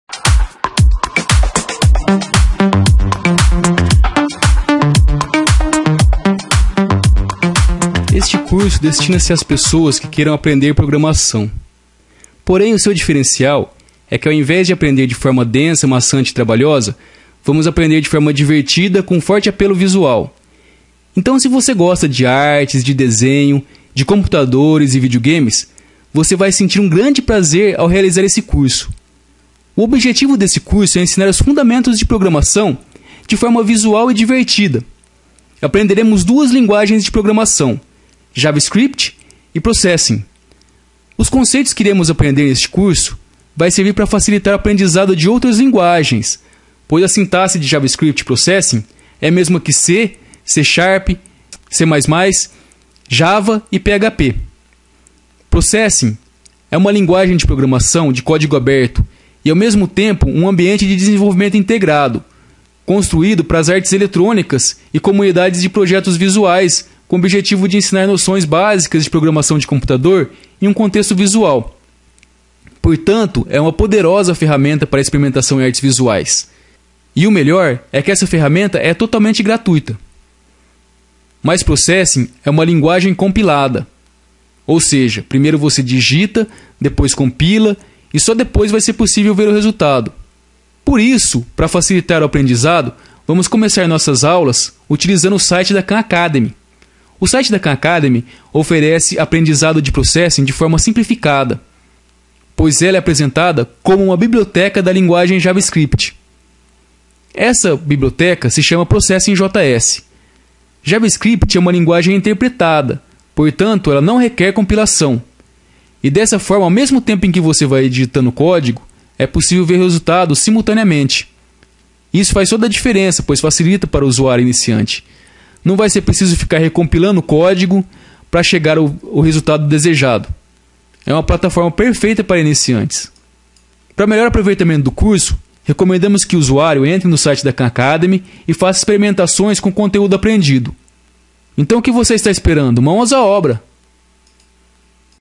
Processing + Javascript - Aula 0 - Introdução
Este vídeo introdutório apresenta o assunto que abordaremos neste curso de javascript + processing. Seu objetivo é ensinar os fundamentos da programação de forma visual e divertida.
 PROCESSING é uma linguagem de programação de código aberto e, ao mesmo tempo, um ambiente de desenvolvimento integrado (IDE), construído para as artes eletrônicas e comunidades de projetos visuais com o objetivo de ensinar noções básicas de programação de computador em um contexto visual. Portanto processing é uma poderosa ferramenta para experimentação em artes visuais. E o melhor é que esta ferramenta é totalmente gratuita.
 JAVASCRIPT é atualmente a principal linguagem para programação em navegadores web. Como é uma linguagem interpretada poderemos visualizar nosso programa funcionando ao mesmo tempo em que digitamos o código. Isso facilita o aprendizado para usuários iniciantes.
 Os conceitos que aprenderemos neste curso vai servir para facilitar o aprendizado de outras linguagens, pois a sintaxe de Javascript e processing e a mesma que C, C++, C #,  Java, PHP entre outras.